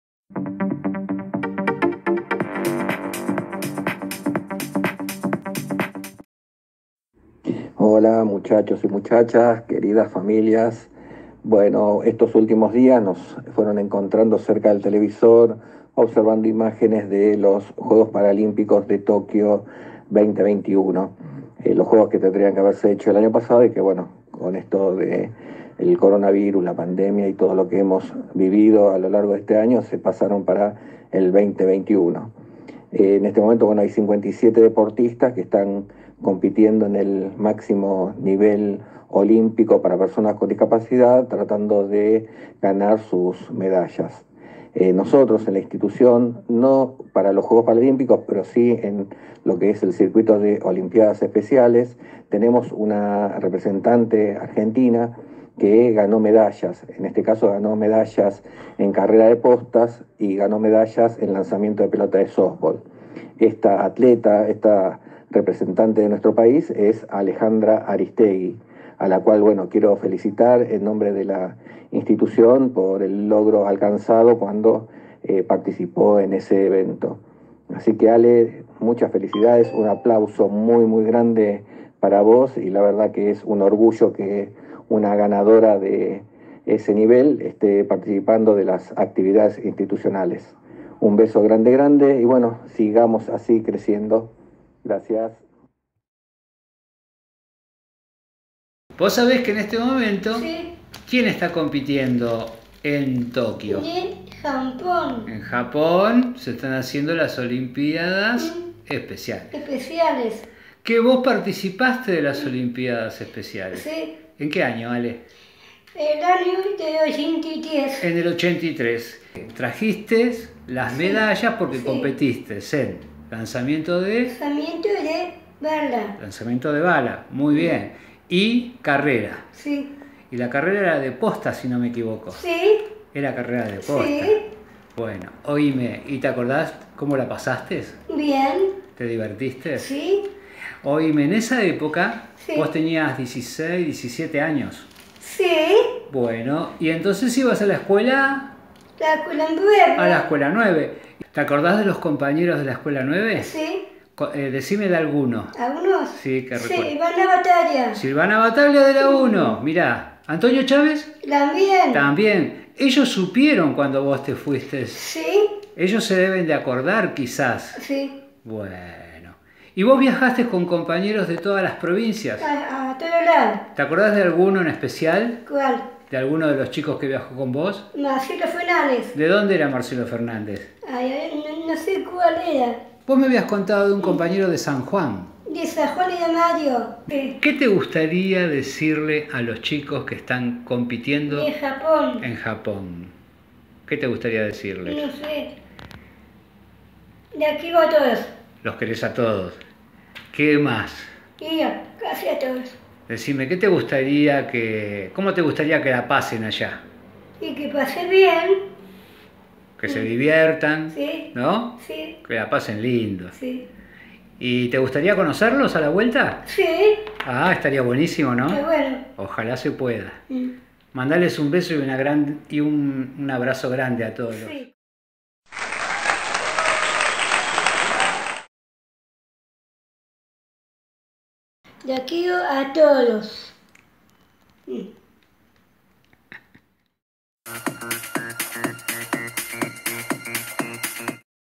ALEJANDRA ARISTEGUI DOBLE MEDALLA EN LAS OLIMPIADAS ESPECIALES 1983 // VIRTUAL // 2021
Alejandra Aristegui doble medalla en las Olimpiadas Especiales 1983

Felicitaciones Totales Ale Aristegui 👏👏👏

Tener el privilegio y placer de representar al país en cualquier instancia deportiva no es sencillo, y ganar medallas, mucho menos.
Tu participación en Olimpiadas Especiales te llevó a lo más alto del deporte en esas competencias logrando medalla en carrera de postas y lanzamiento de pelota de softbol.

Hoy, en otro circuito de competencia, la Delegación Paralímpica Argentina cuenta con 57 deportistas compitiendo en Tokio en lo más alto de las competencias para personas con discapacidad.
Esperemos que muchos de ellos puedan lucir sus medallas Paralímpicas como pudiste hacerlo vos en las Olimpiadas Especiales de 1983.

Gracias Alejandra!! para nuestra Institución es un orgullo enorme que compartas tus medallas con nosotros.

Osvaldo Barberio, Prof. Educ física